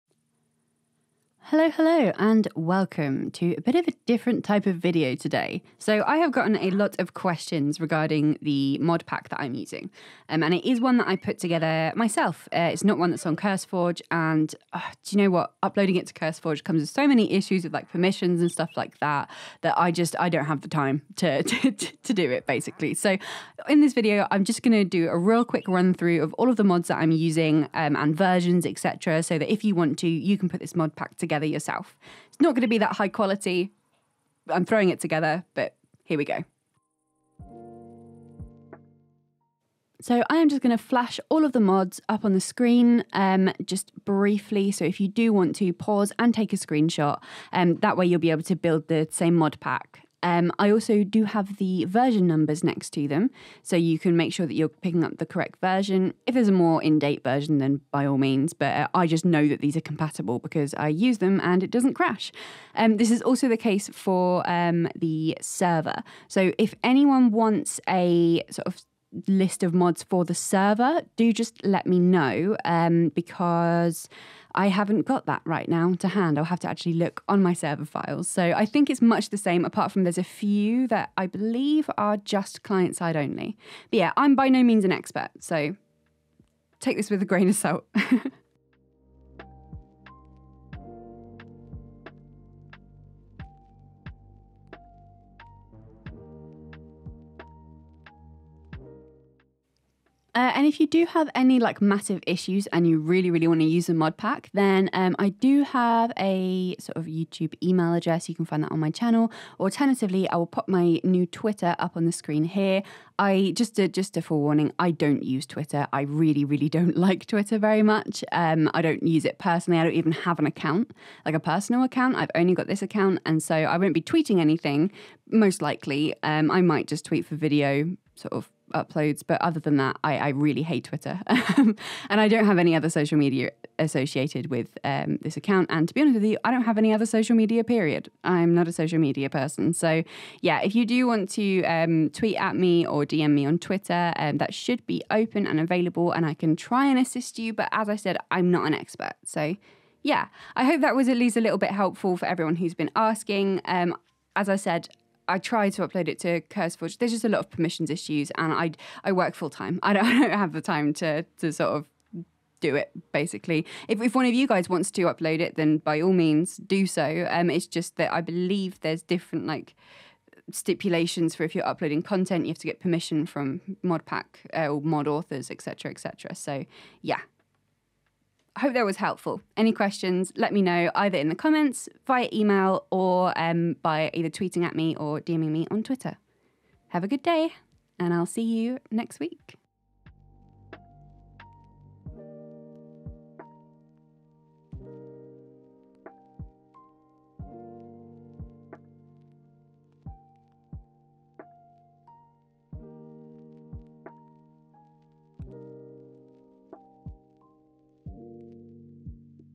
My Minecolonies Modlist [CC]
Just a real quick video showing my modlist for my minecolonies series!

As I said, I'm no expert and so I'm probably not going to be able to solve most issues with it as it took me a while to make the pack work!

It's best to create a modpack on the curseforge app because 1. that's how I made it work, and 2. If I've forgotten any dependencies, it'll automatically add them!